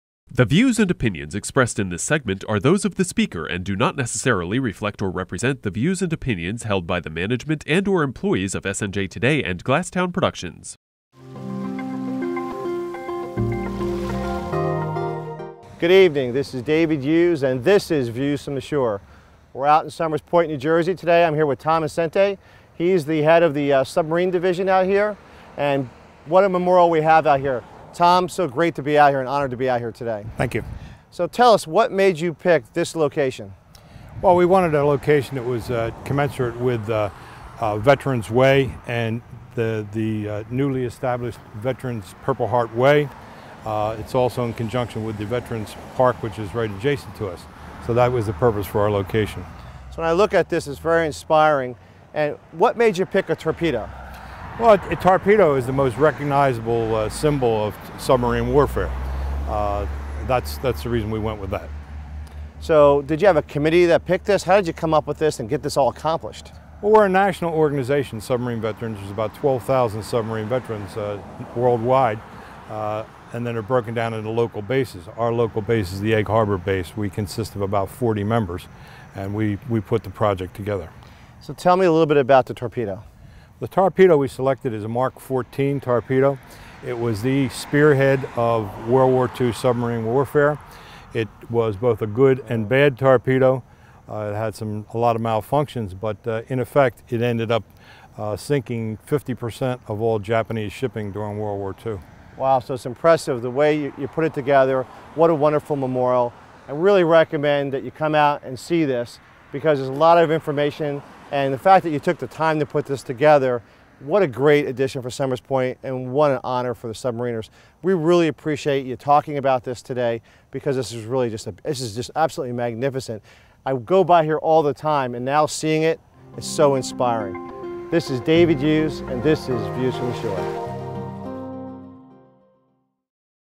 Views From The Shore - Tom Innocente - Ep. 90
This week David Hughes is on location with Egg Harbor US Submarine Veterans Base Commander Tom Innocente to talk about the Submarine Veterans Memorial in Somers Point.